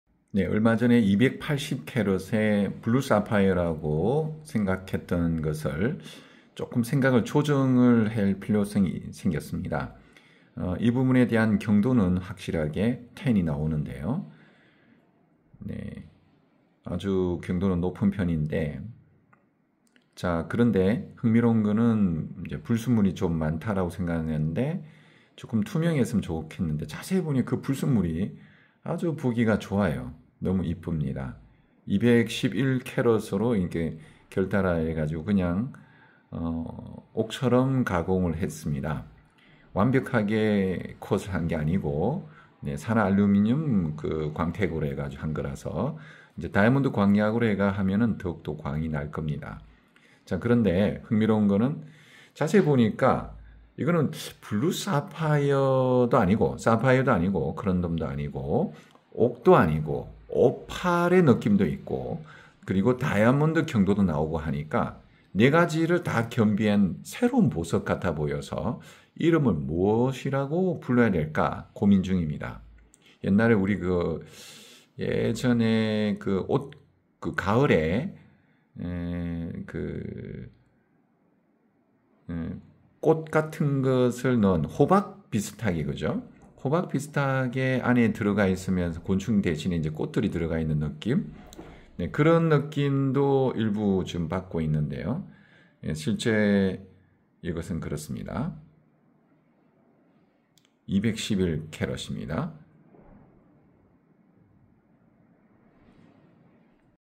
포항에서 발견된 다이아몬드 경도에 오팔 사파이어 옥의 특징Characteristics of Opal Sapphire Jade in Diamond Hardness,in Pohang
포항에서 발견된 새로운 보석 이름은 무엇으로 정할까요? 지나번에 블루 사파이어로 여겼던 것이 실제로 안을 들여다보니 다른 성질의 것이었습니다. 경도는 9.5에서 10 다이아몬드 경도가 나오면서 오팔같은 느낌에 사파이어보다 더 경도가 높고 옥같은 전성도 있으면서 안의내포물이 단순 불순물로 여기기에는 너무 아름다운 미적감각을 갖고 있어서 마치 에델바이스나 호박처럼 안에 내포물이 아름답습니다. 좀더 컷팅을 이쁘게 모양을 잡으면 제대로 멋있는 보석이 될 것 같습니다. 이름을 무엇으로 정할까요? 포항에서 나온 보석입니다.

광협보석갤러리카페 한국광협보석감정원 앵두금성분분석소
What will be the name of the new gem discovered in Pohang? What I thought was a blue sapphire the other day actually turned out to be of a different nature when I looked inside. The hardness ranges from 9.5 to 10 diamond hardness, making it feel like an opal, harder than sapphire, and malleable like jade. However, it has an aesthetic sense that is too beautiful to consider the inclusions inside as simple impurities, making the inclusions inside beautiful like edelweiss or amber. I think it would become a really cool piece of jewelry if it were cut and shaped more beautifully. What should we name it? This is a gem from Pohang.

Gwanghyup Jewelry Gallery Cafe Korea Gwanghyup Gem Appraisal Institute Cherry Gold Component Analysis Laboratory